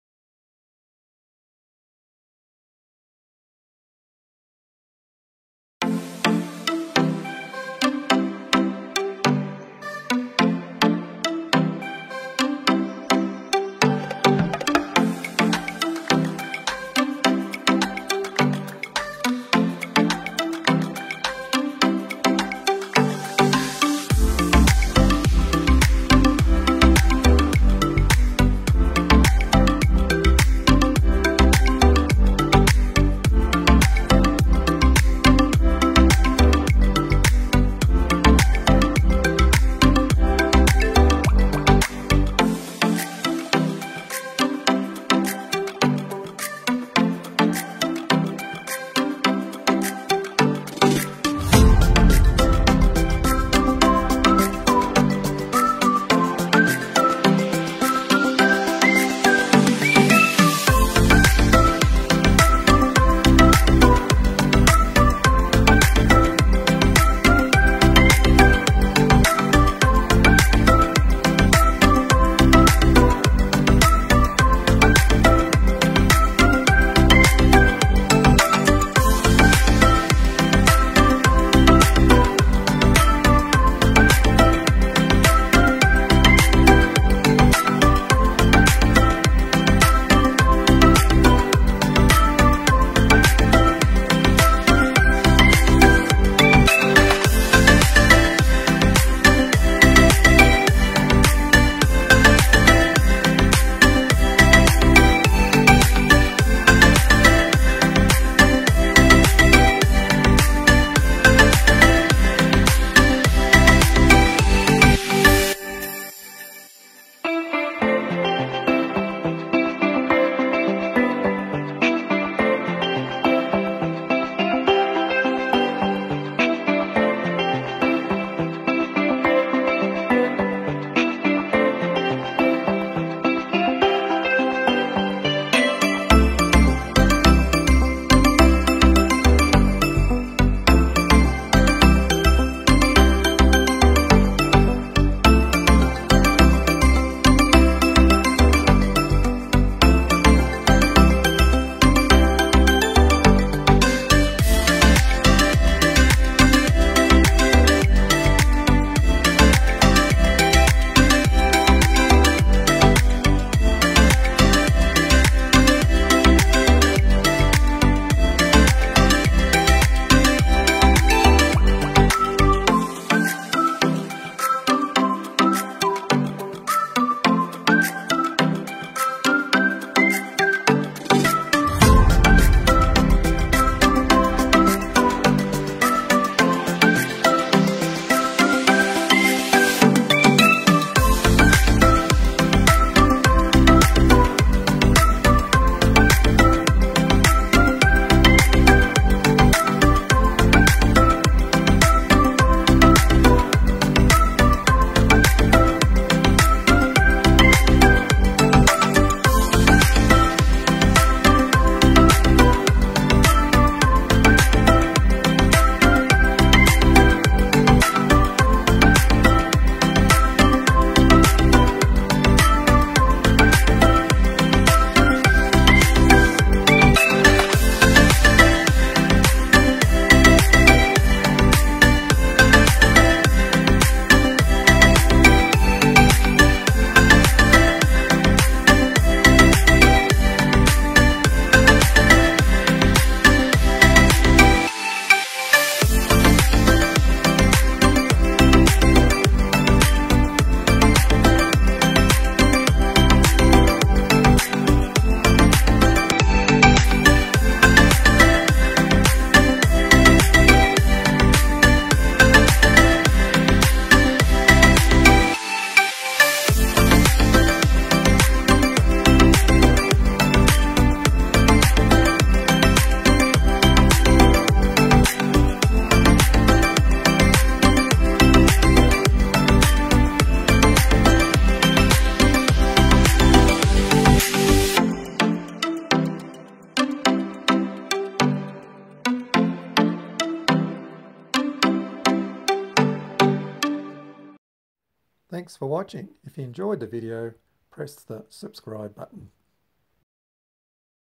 Bergen Funicular to Mount Floyen, Norway
28 July 2023
Ride Bergen funicular railway one way to the top of Mount Floyen (320m), see views over Bergen from viewpoint. Walk to Revurtjern Lake and Lake Skomakerdiket. Walk back down to Bergen city centre.